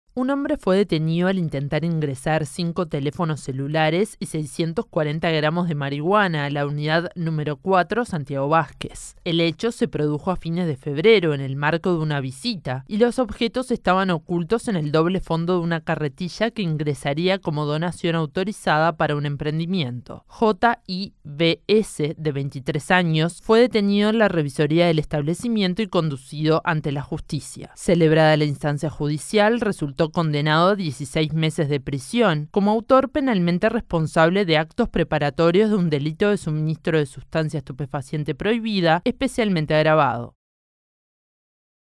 Detenido al intentar ingresar celulares y marihuana a la Unidad Nº 4 (Santiago Vázquez)
Los 5 teléfonos y los 640 gramos de droga estaban ocultos en el doble fondo de una carretilla que ingresaría al establecimiento como donación.